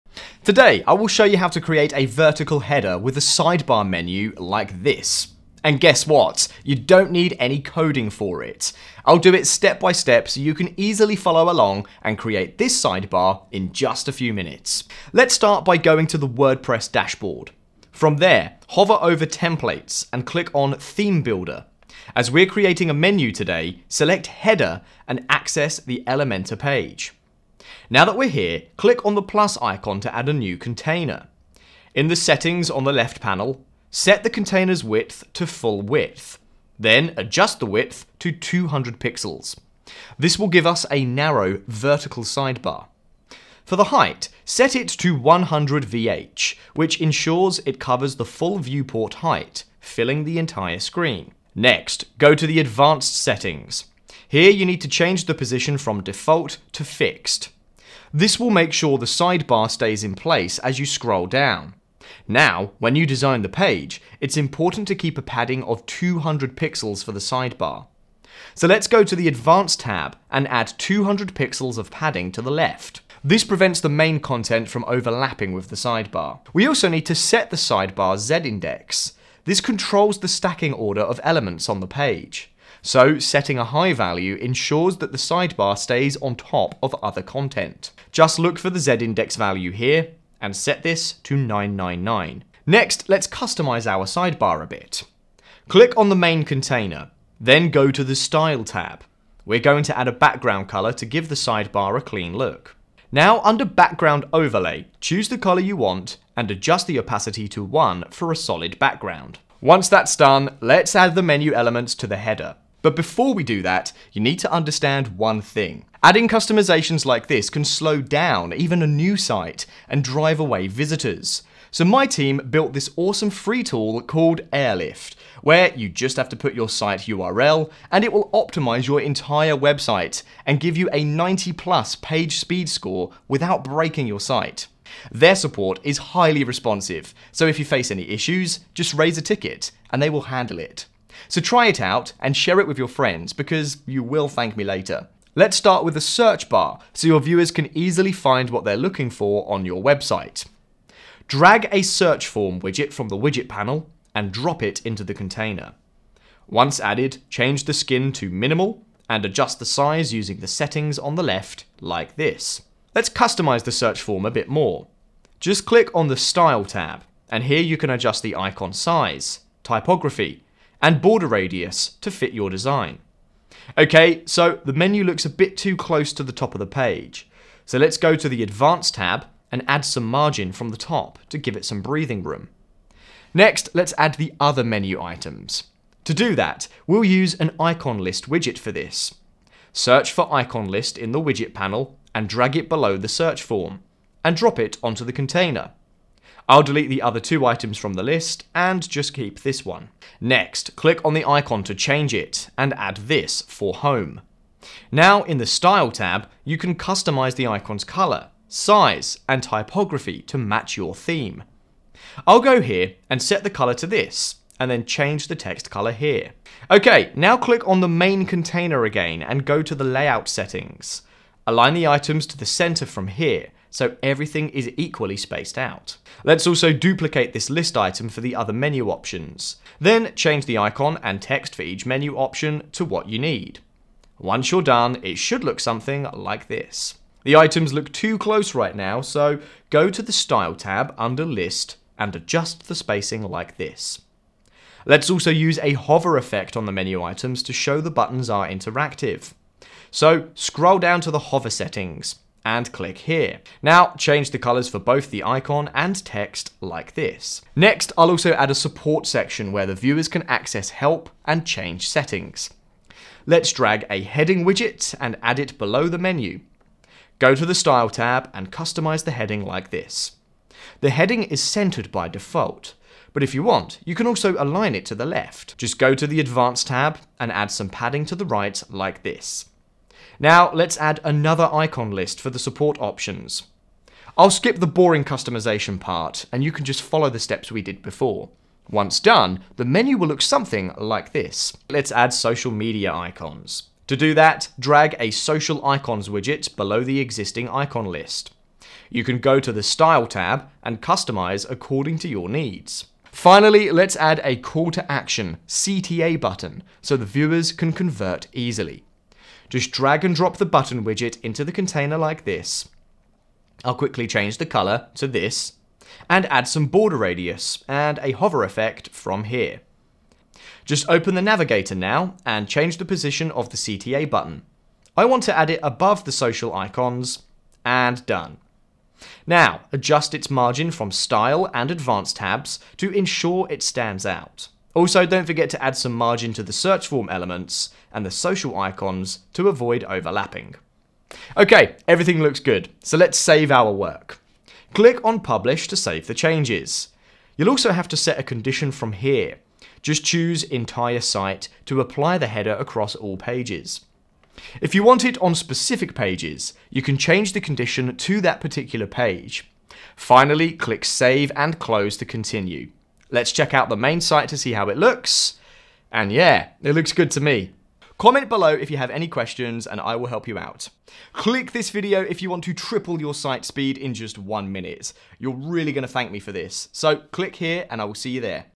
Elementor - Vertical Header with Sidebar Menu
⭐ Giveaway: Our 100% FREE plugin makes any site 3X faster In this video, I'll show you how to create a fixed vertical header with a sidebar menu in WordPress, all without writing a single line of code.

We'll use Elementor's Theme Builder and walk you through setting up a fixed sidebar and adding essential elements like a search bar, menu items, and social icons. You'll also learn how to properly space your elements, insert a call-to-action button, and ensure your sidebar looks great on all pages.

This video is inspired by Web Squadron, so make sure to follow him for awesome content!

00:00 Create a custom sidebar like this
00:15 Create a layout for the menu
01:02 Important, don't miss!
01:32 Customize sidebar
02:25 Add menu elements
04:16 Add support section
05:00 Add CTA button
05:43 Publish the sidebar